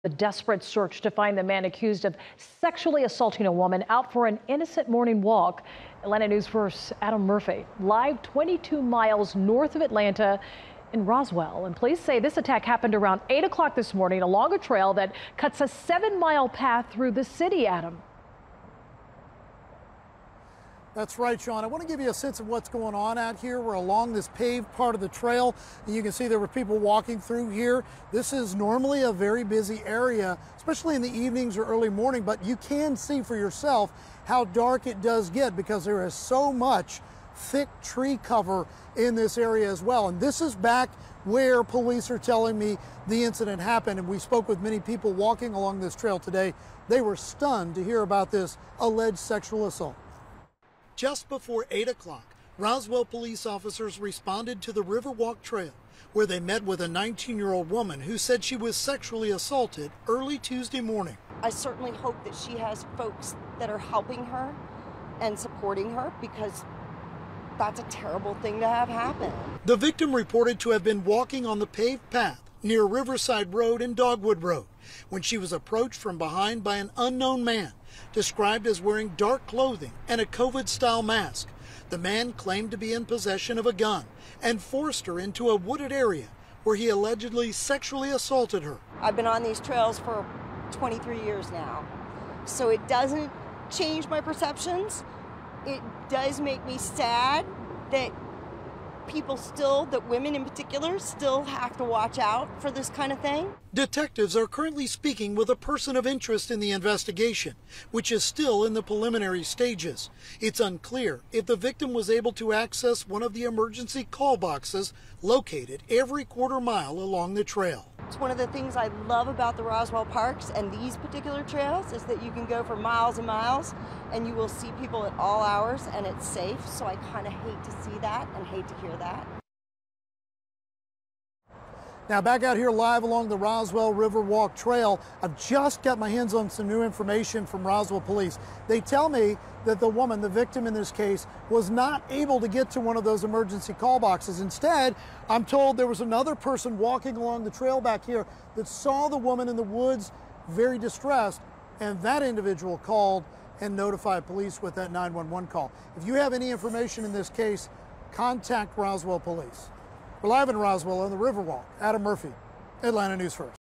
Teen says she was sexually assaulted along trail in Roswell, police say
The incident happened Tuesday in a wooded area near Riverwalk Trail just before 8 p.m., close to Riverside Road and Dogwood Road.